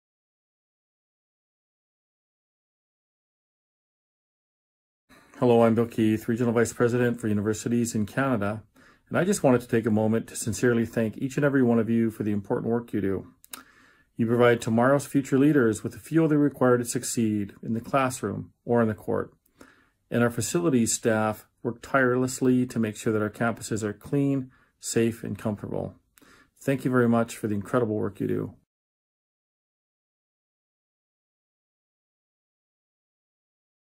EmployeeAppreciationWeek
Celebrating Sodexo Canada's Employee Appreciation Week!
A Thank You message from Bill Keith, Vice President for Education, Sodexo Canada